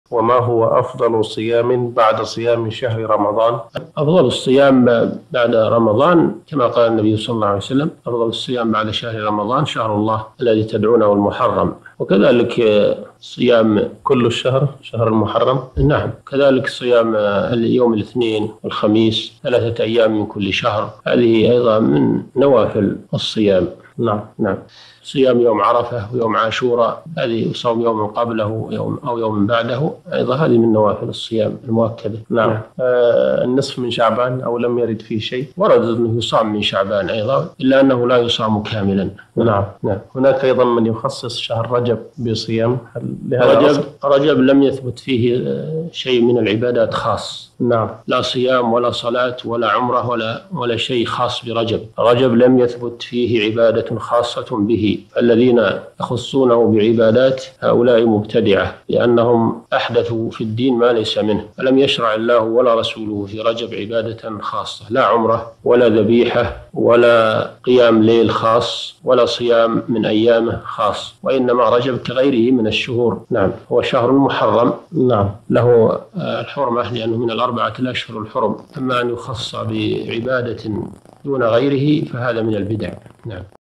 ماهو أفضل صيام بعد صيام شهر رمضان؟ الشيخ صالح بن فوزان الفوزان
نص السؤال:
ماهو أفضل صيام بعد صيام شهر رمضان؟

السيرة الذاتية :
للاشتراك في قناة الشيخ صالح الفوزان

رئيس الفريق
لمشروع كبار العلماء